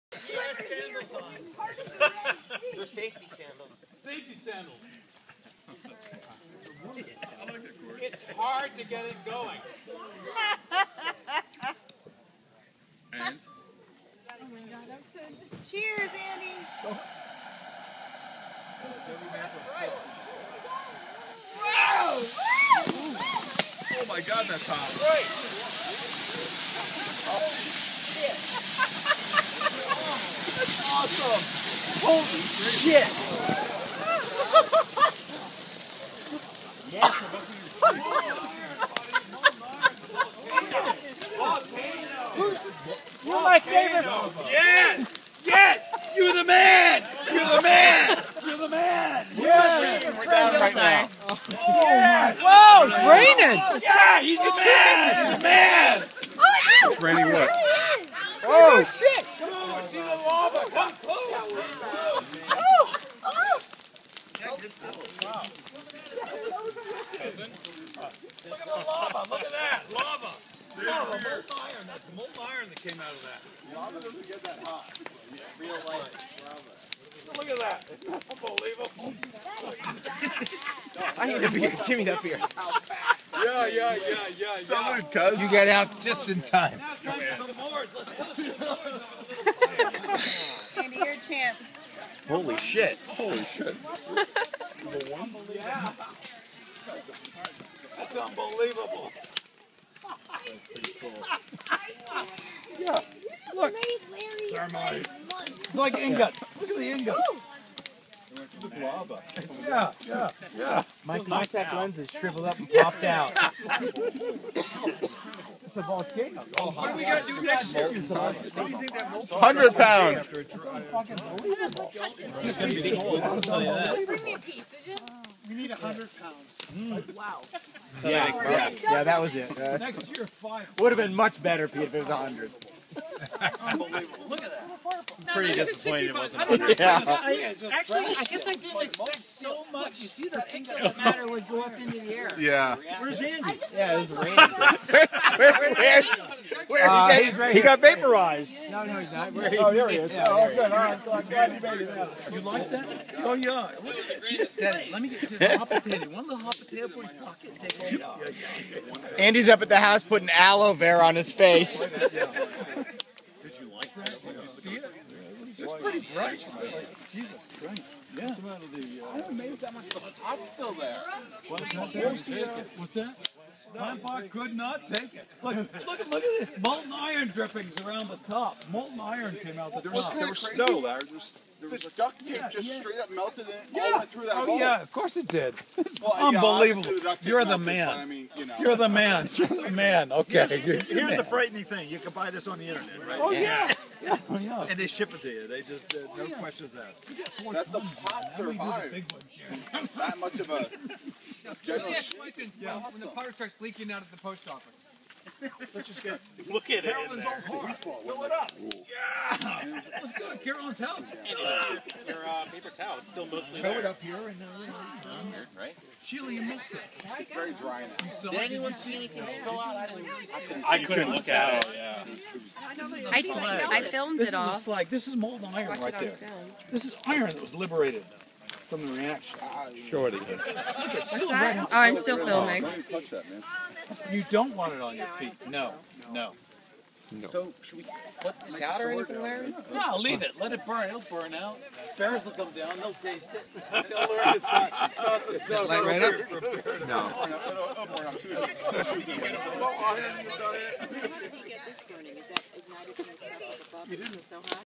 60 lbs of thermite, a clean getaway, a rain of aluminum and iron beads, and a whole lot of fun!
Celebrating the 2013 Summer Solstice with some thermite.